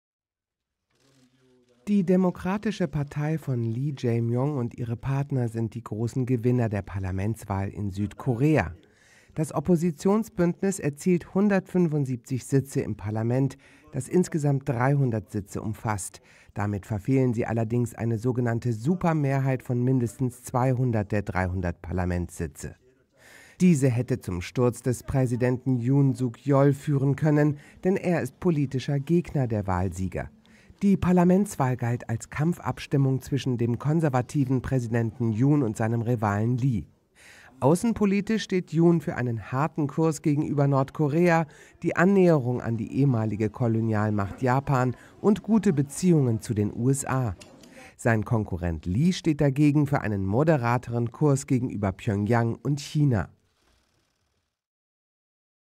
Deutlicher Sieg der Opposition bei Parlamentswahl in Südkorea | AFP
Die demokratische Partei (DP) von Lee Jae Myung und ihre Partner sind die großen Gewinner der Parlamentswahl in Südkorea. Das Oppositionsbündnis erzielt 175 Sitze im Parlament, das insgesamt 300 Sitze umfasst – damit verfehlen sie allerdings eine sogenannte "Super-Mehrheit" von mindestens 200 der 300 Parlamentssitze.